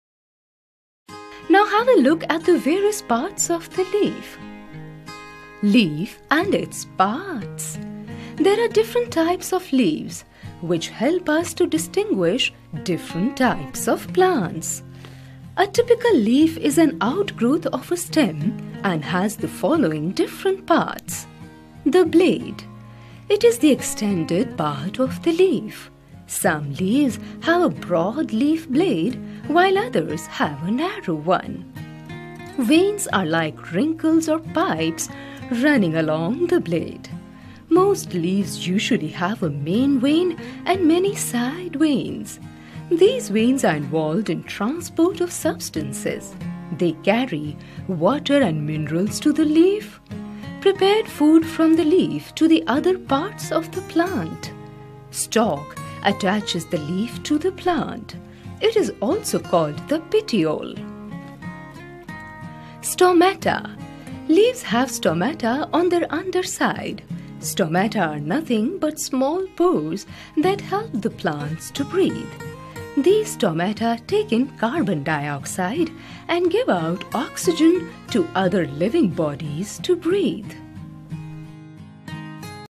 Science - Plants our Green Friends Part 2 - Leaf and its part - CBSE Class 4 - Science IV
Leaves are important because plants make their own food in leaves. The leaves contain a pigment called chlorophyll.